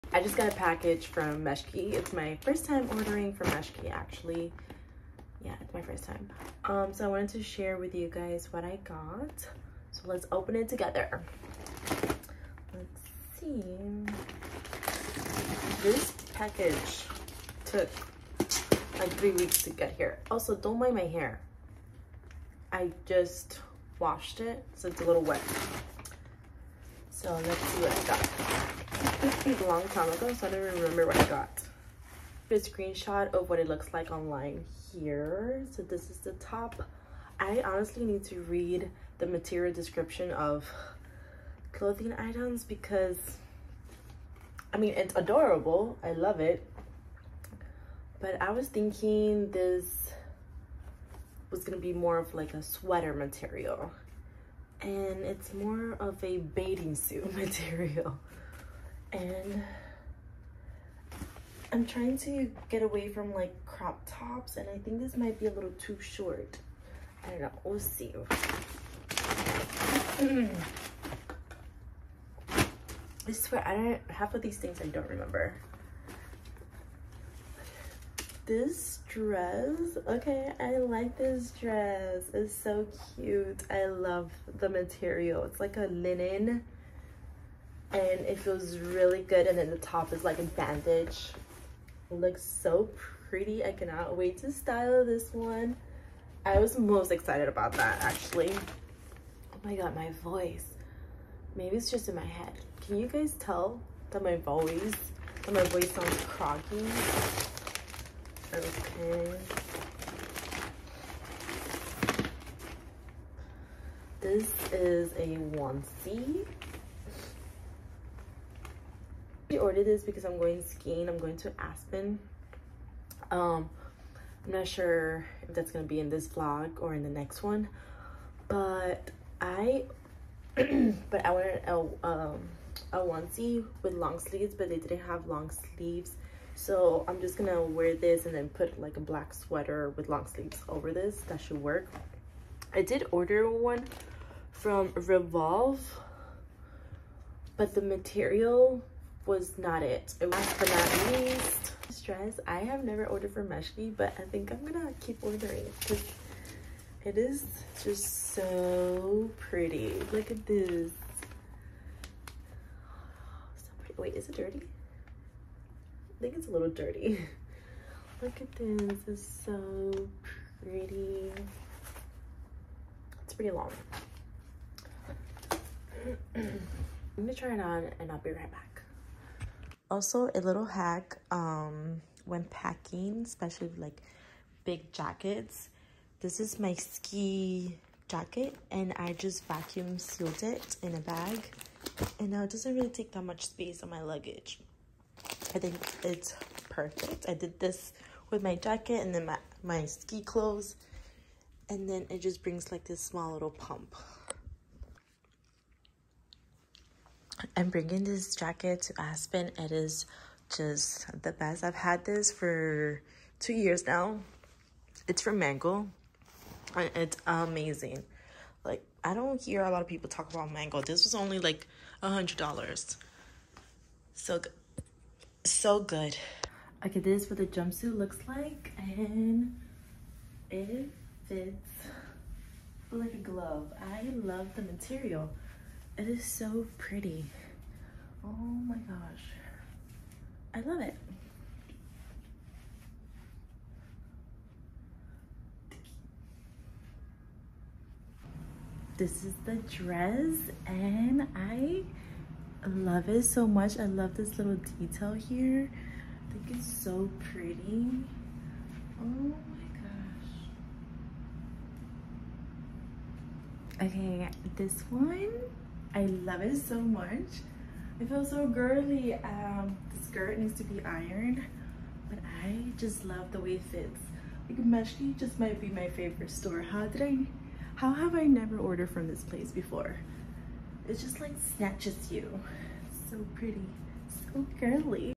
First time ordering Meshki + try on haul + packing for Aspen!
Meshki Try on Haul + Packing for Aspen!
Meshki clothes order try on haul review, first time purchasing from meshki and it did not disappoint! Also, packing for aspen :)

LINKS:
✨ Amazon Storefront